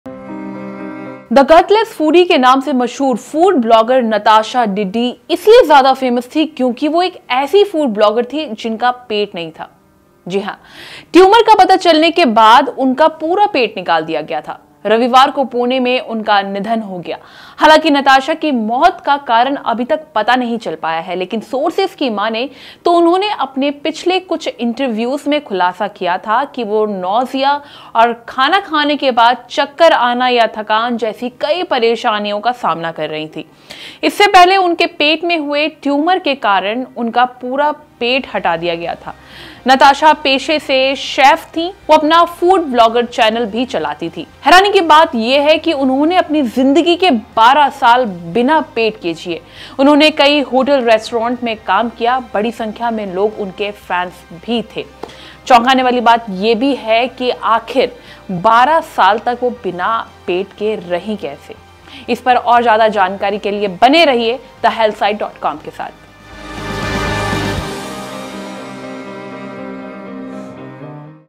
The Gutless Foodie! | Natasha Diddee, the food blogger who lived without a stomach,dies in Pune
The Gutless Foodie aka Natasha Diddee: ‘Thegutlessfoodie' के नाम से मशहूर फूड ब्लॉगर Natasha Diddee इसलिए ज्यादा Famous थीं क्योंकि वह एक ऐसी food blogger थी जिनका पेट नहीं था। Tumor का पता चलने के बाद उनका पूरा पेट निकाल दिया गया था। रविवार को Pune में उनका निधन हो गया। हालांकी Natasha Diddee की मौत का कारण अभी तक पता नहीं चल पाया है, लेकिन सोर्सस की मानें तो उन्होंने अपने पिछले कुछ इंटरव्यूज में, खुलासा किया था कि वह Nausea और खाना खाने के बाद चक्कर आना या थकान जैसी कई परेशानियों का सामना कर रही थीं.